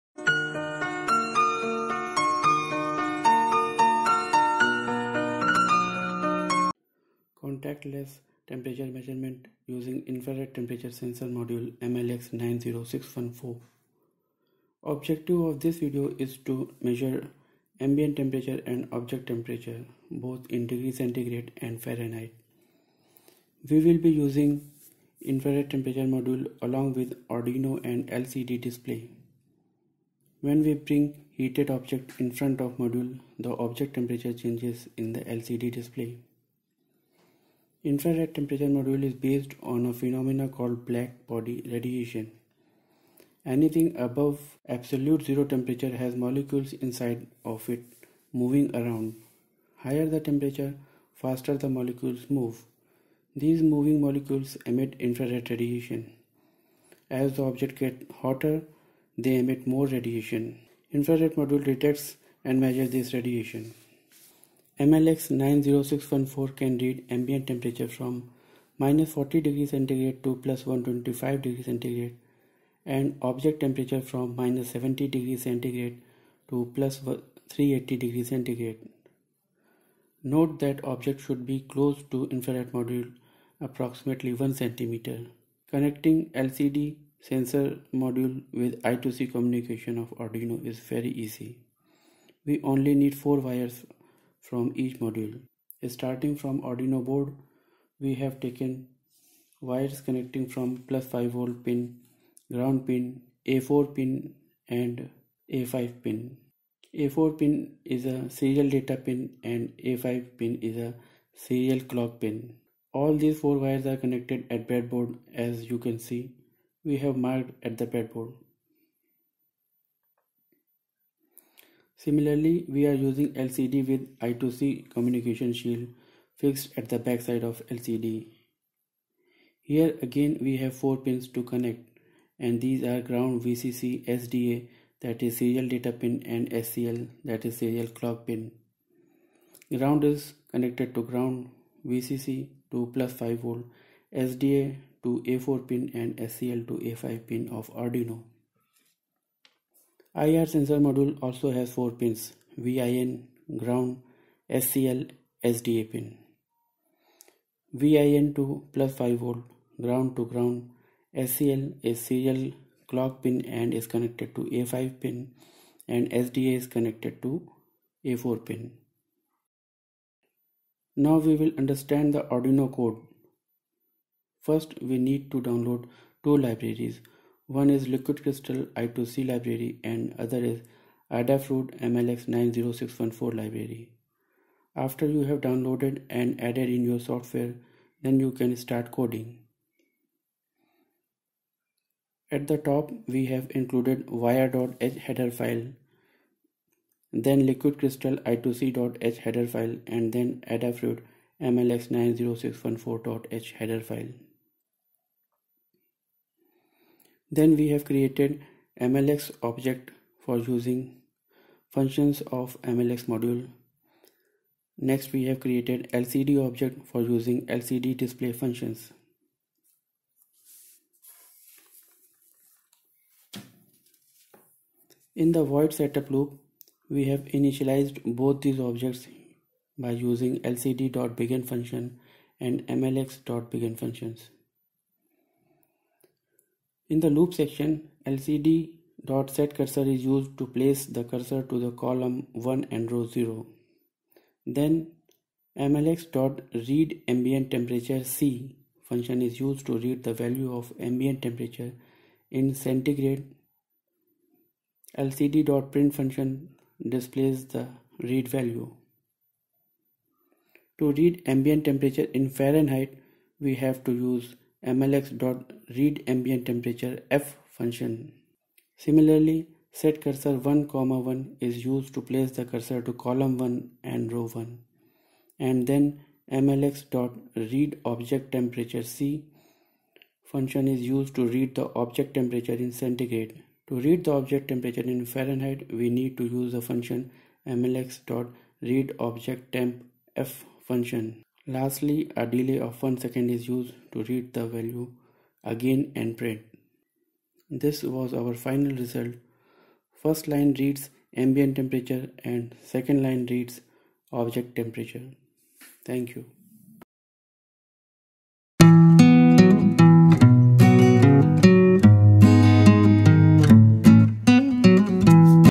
Contactless Temperature Measurement
This tutorial video demonstrates how to use Infrared Temperature module MLX90614 to measure ambient temperature and object temperature. Along with IR temperature sensor module we have used Arduino UNO board and LCD for display of temperature data.